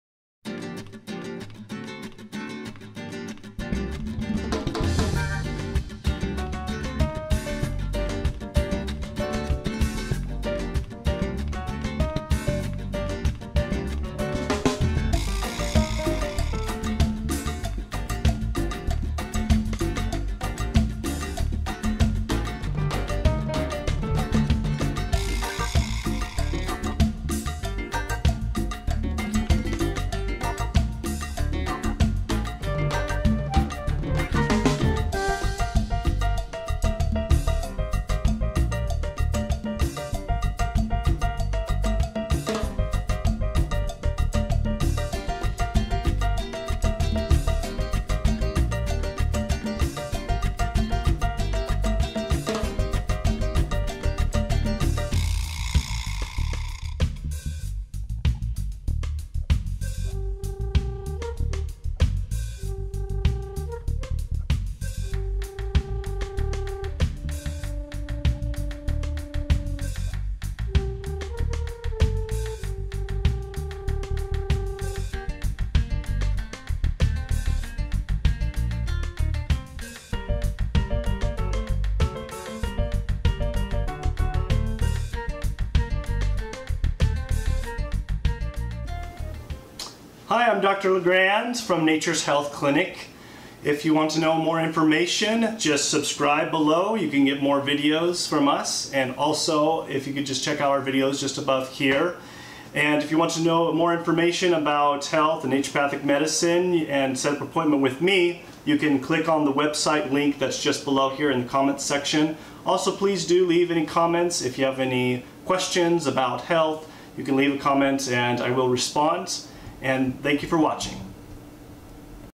Easy and Delicious Guacamole Recipe
Try my easy and delicious guacamole recipe. Not only for just a chip dip but good for sandwich spreads and salads.

Dr. LeGrand is a Naturopathic Doctor that Graduated from Bastyr University.